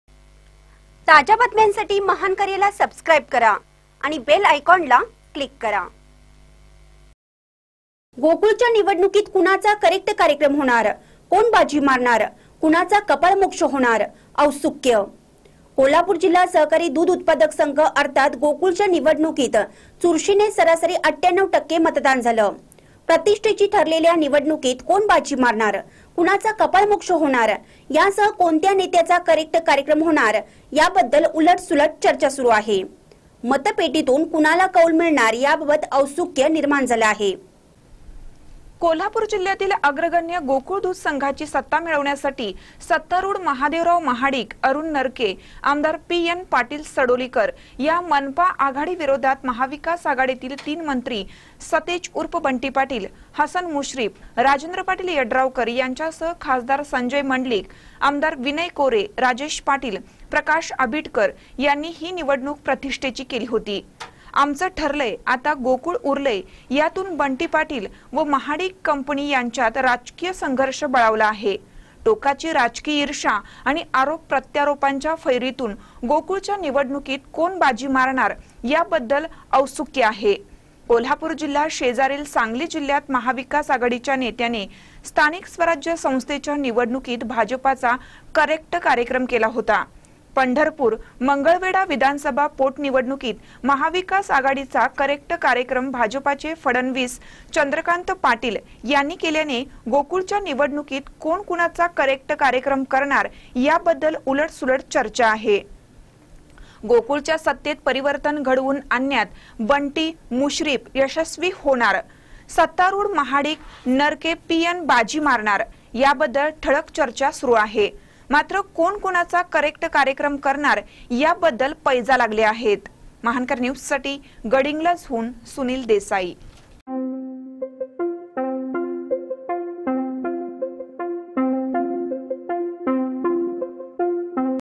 गोकुळच्या निवडणुकीत कुणाचा करेक्ट कार्यक्रम होणार...? कोण बाजी मारणार...?
गोकुळच्या निवडणुकीत कुणाचा करेक्ट कार्यक्रम होणार?, कोण बाजी मारणार? कुणाचा कपाळमोक्ष होणार? औत्सुक्य!